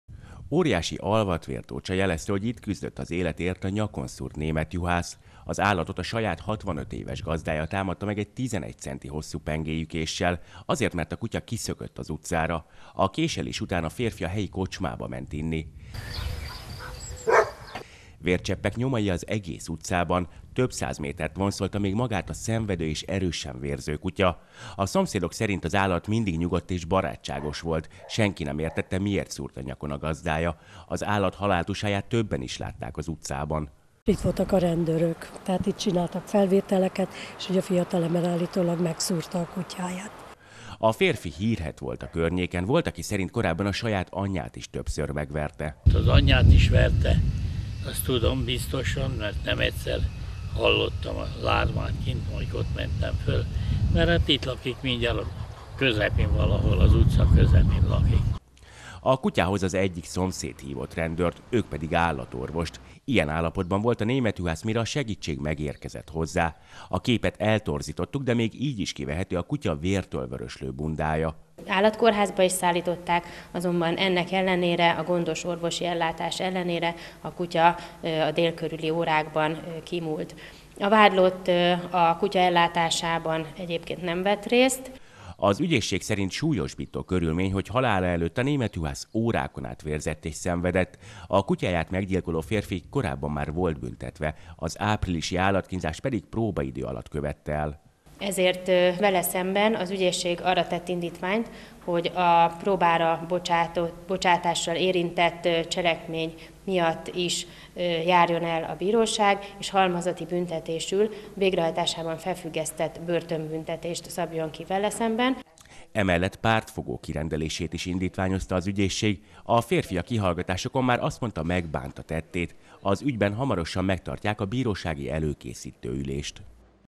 Állatkínzás – felfüggesztett börtönt kér az ügyész a kutyáját nyakon szúró férfira20191204
Nyakon szúrta, majd órákon át hagyta szenvedni a németjuhászát egy férfi Nagymányokon! Az állat még a szakszerű orvosi ellátás ellenére sem élte túl a kegyetlen támadást. Az ügyben most emelt vádat az ügyészség.